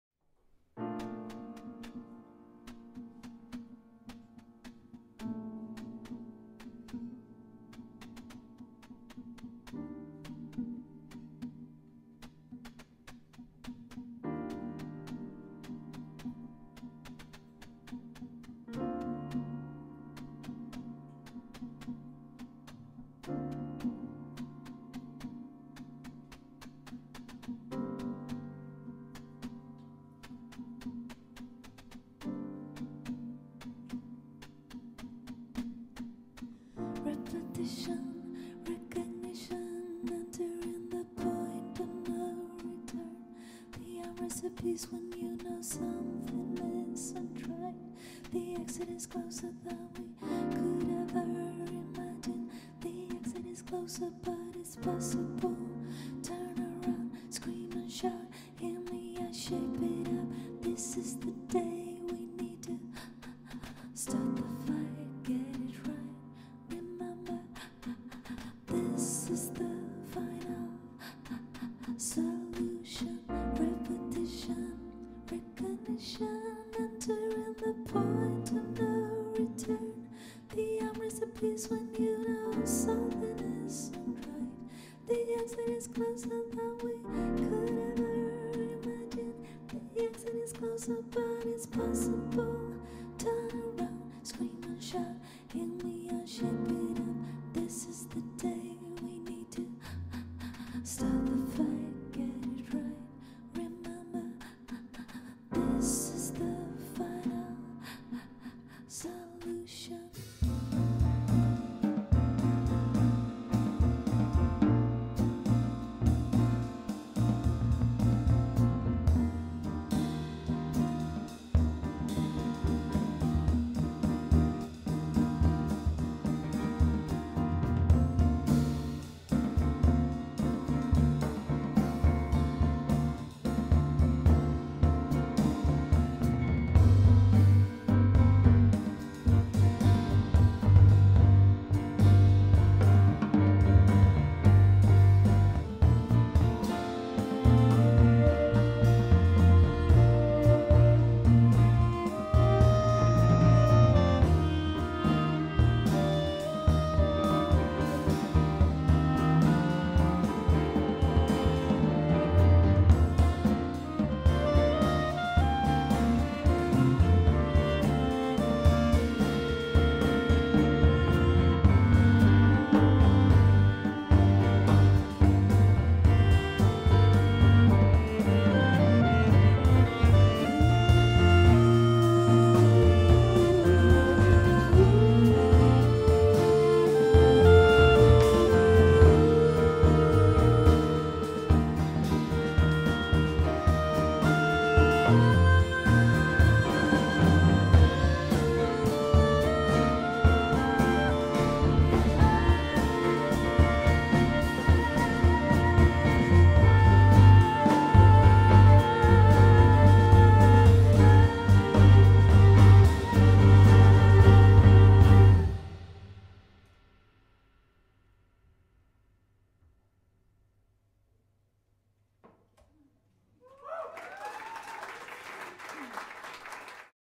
7518 - Thomas Backman LIVE Fasching Stockholm 230126
Thomas Backman: Reeds
Josefine Lindstrand: Lead vocals, synthesizer, piano
Cecilia Linné: Cello, guitar, backing vocals
David Lindvall: Bass, synthesizer
Martin Öhman: Drums, electronics